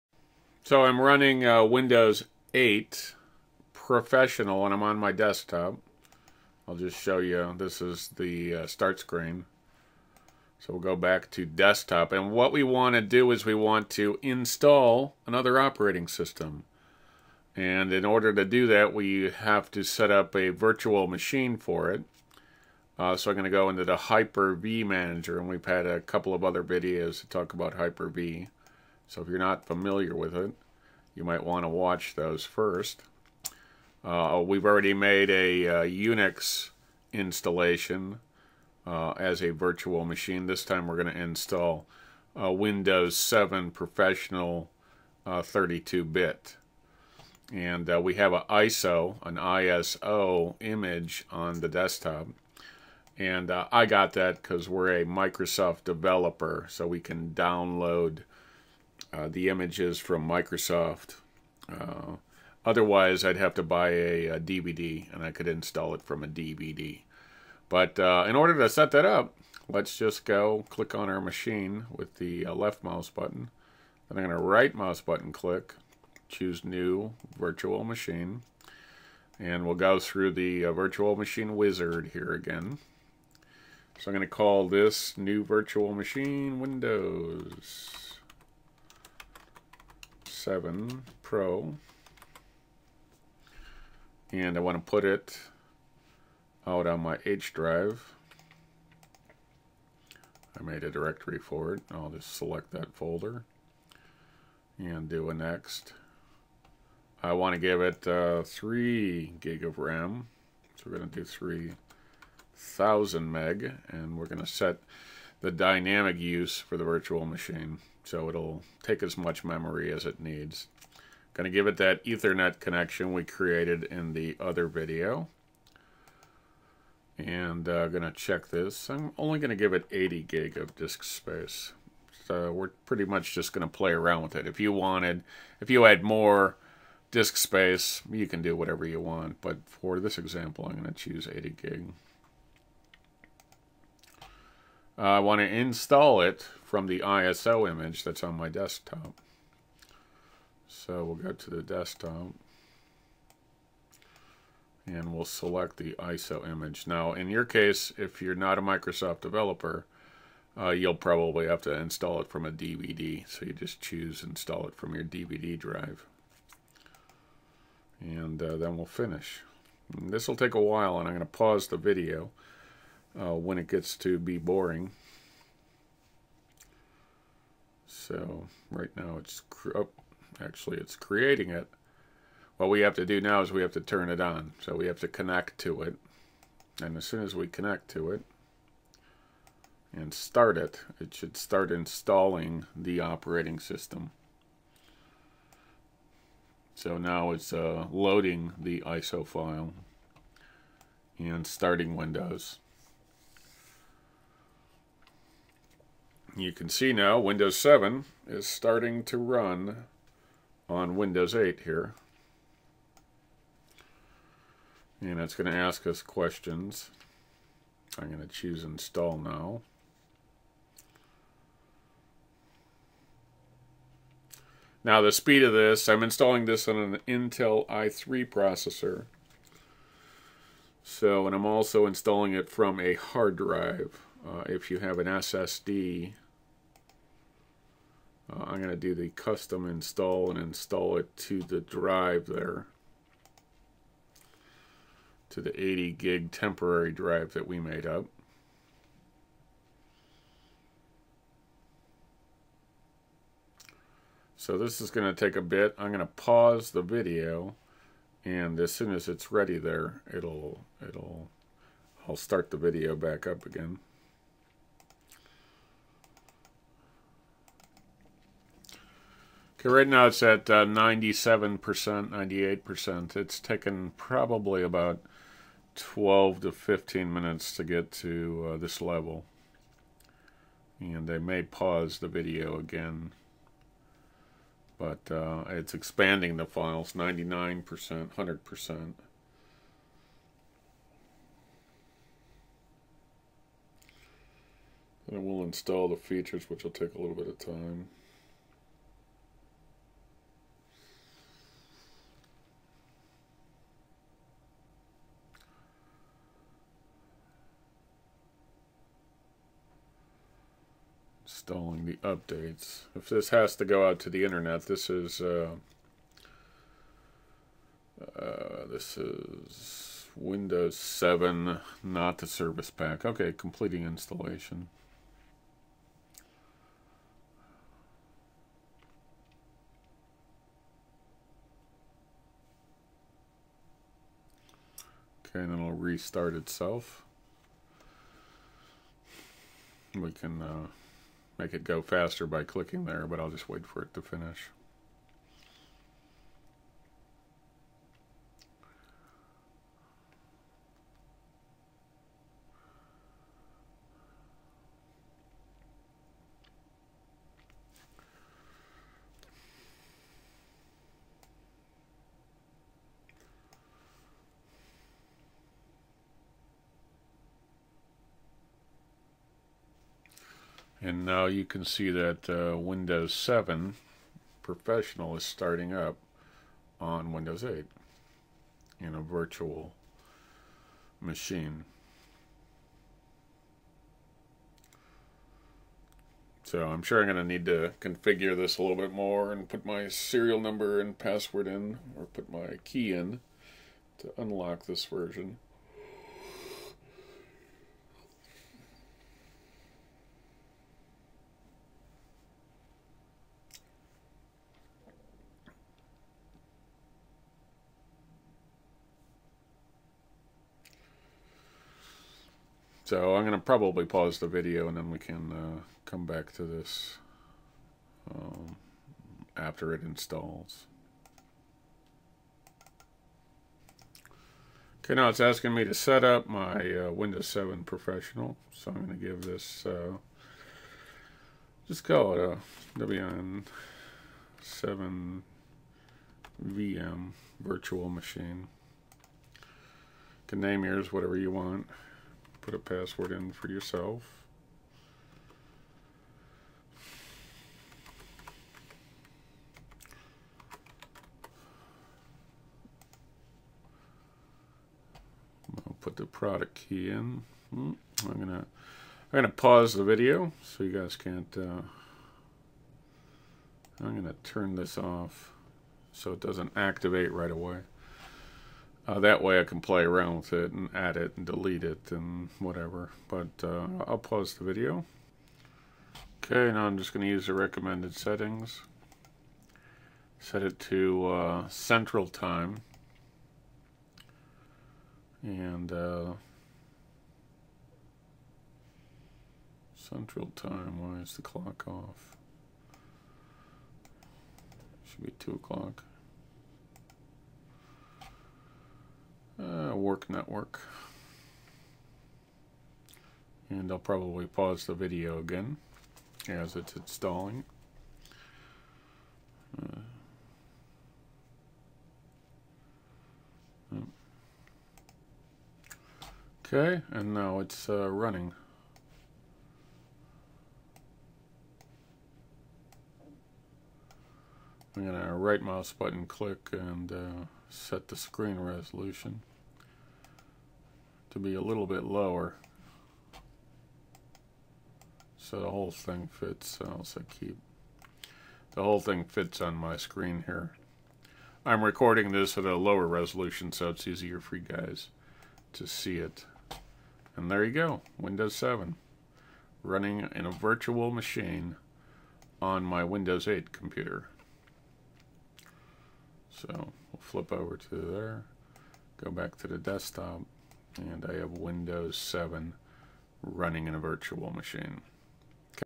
Install Windows 7 & Run on Windows 8 Pro with Hyper-v on a virtual machine
If I have to learn ... why not teach you too series.   One of the great things with windows 8 is its ability to run multiple operating systems on your same computer at the same time.  Hyper-v is a free application on windows 8 pro and windows 8 enterprise.

This shows how to install windows 7 and get it running on your windows 8 pc at the same time.